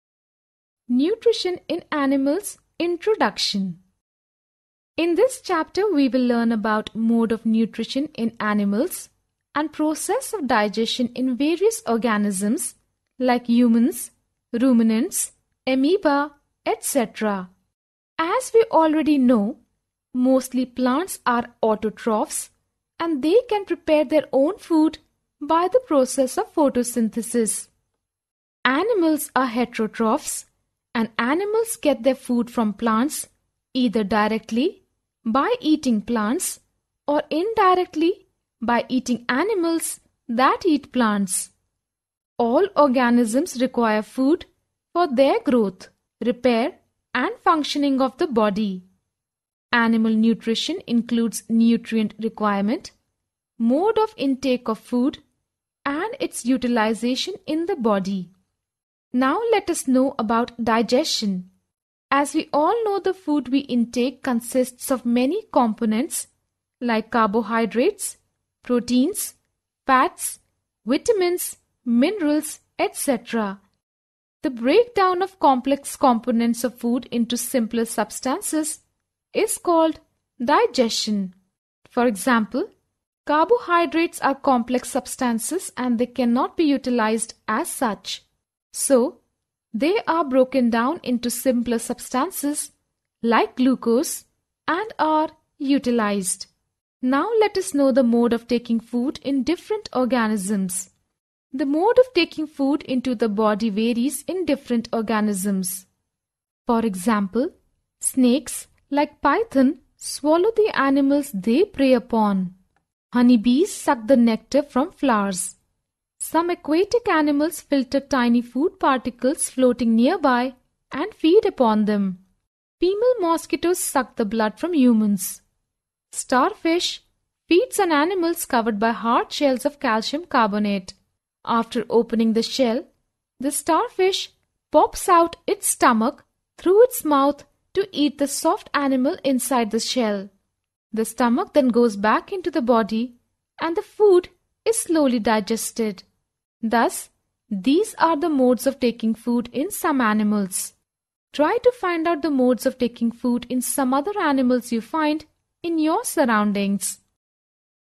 NCERT CBSE Class 7 Science Chapter 2 Nutrition in Animals Part 1
In this video “Introduction to Nutrition in Animals” has been explained according to NCERT CBSE Class 7 Science Chapter 2 “Nutrition in Animals”. for complete free access to the course